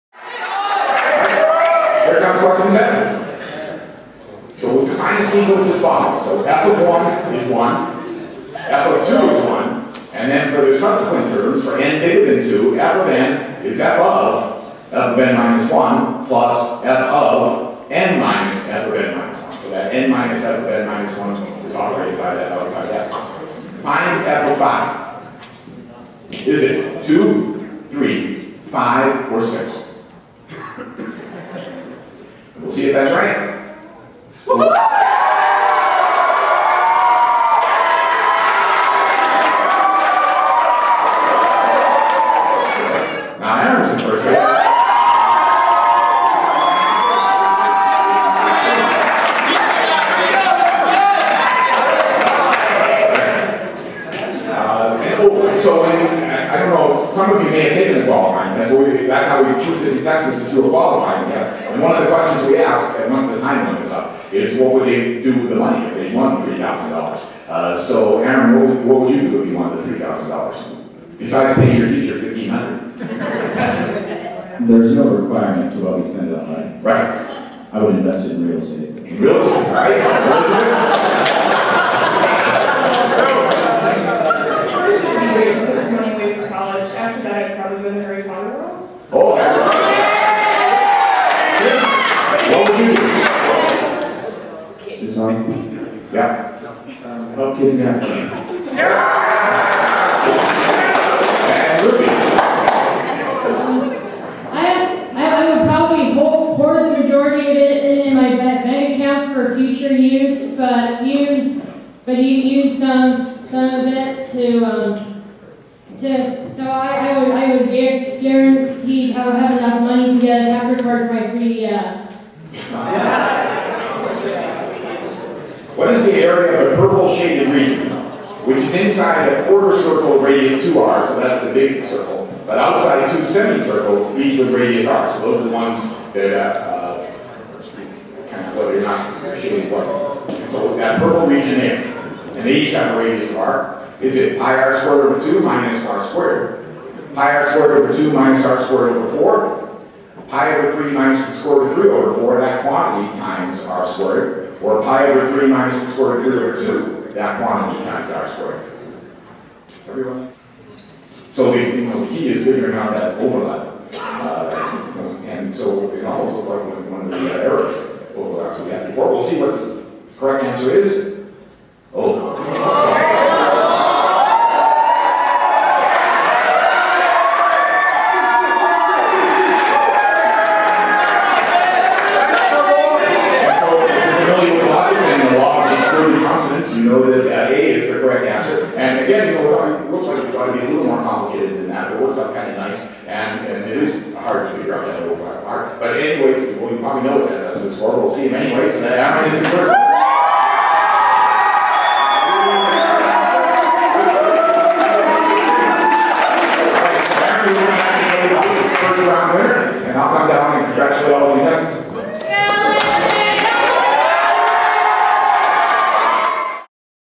WWTBAM Arnold Ross 2014 - Game 1 Part 3
The last two questions from game one of Who Wants to Be a Mathematician at the 2014 Arnold Ross Lecture in Salt Lake City.